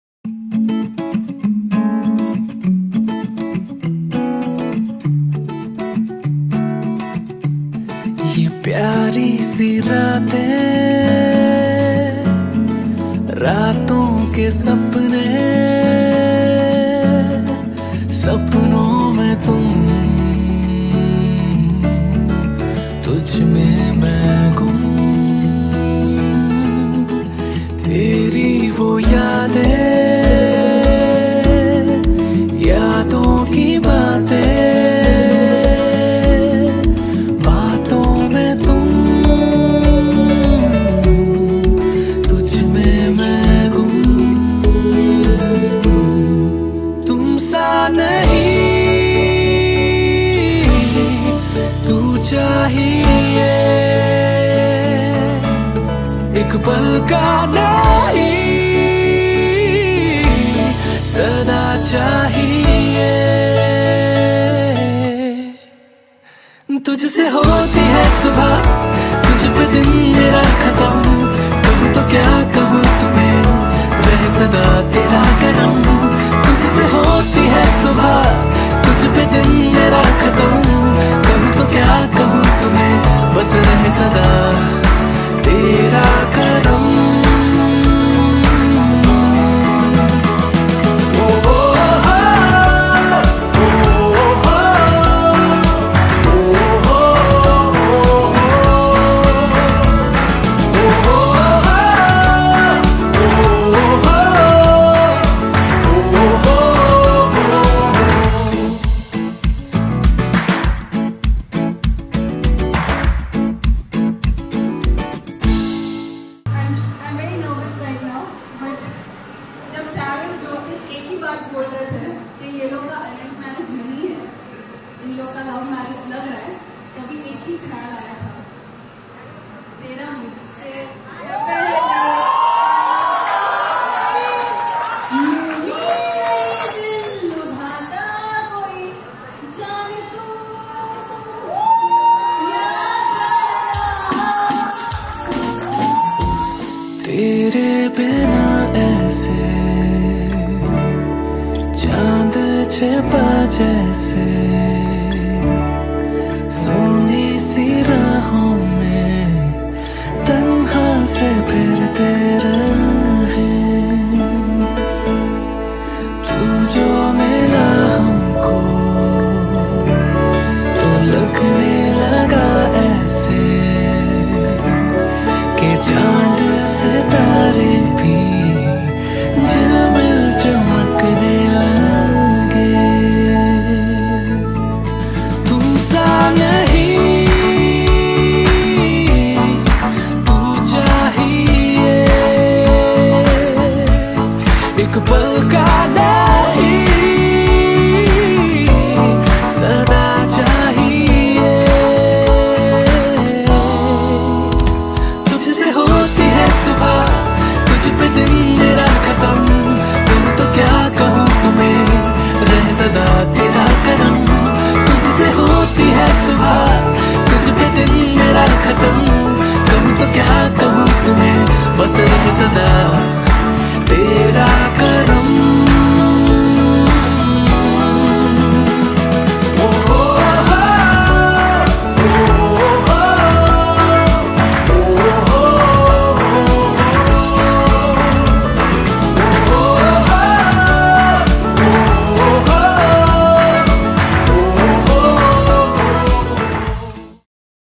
Ishita x Tejas || IshqkaTej || Official Ring Ceremony Film || TBC PRODUCTIONS
Here's Coming Engagement Highlight Video For You Guys !!
We Hope You Like The Video !!
Give It A Big Thumbs UP

Video By -  The Branded Clicks
We Conduct All Kind Of Photography Events !!